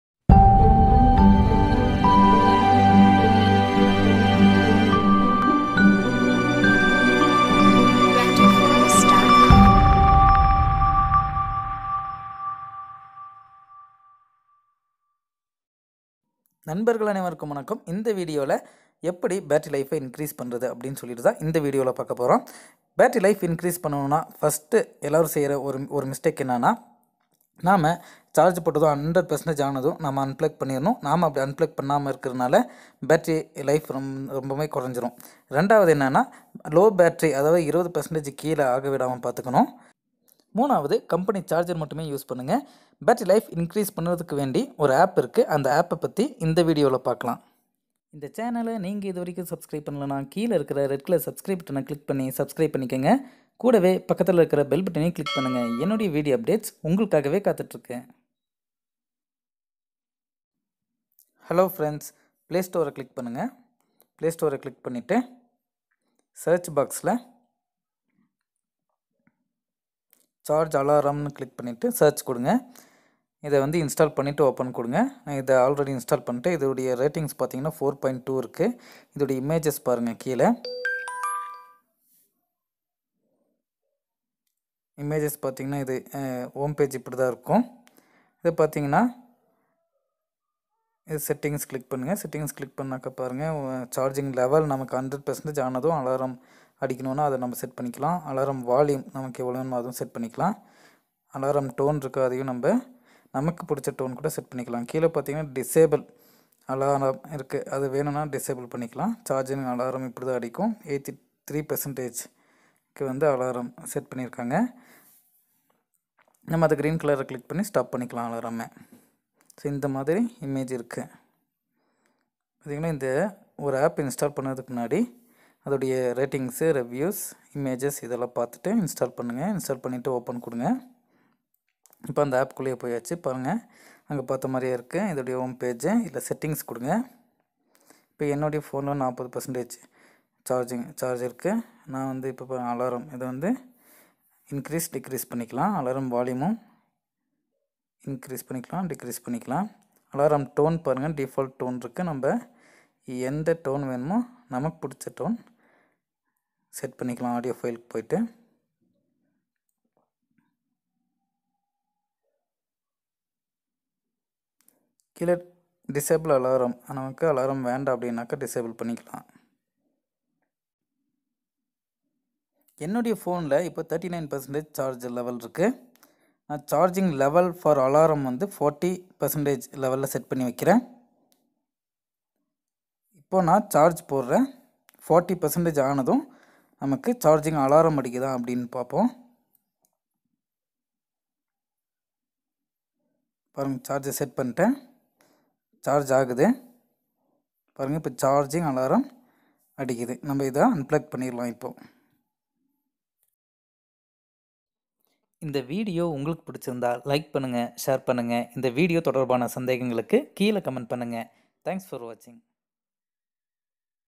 How to Increase Battery Life on Android in Tamil|Tamil Tech Animation
In this video i will show you how to increase battery life on android in tamil.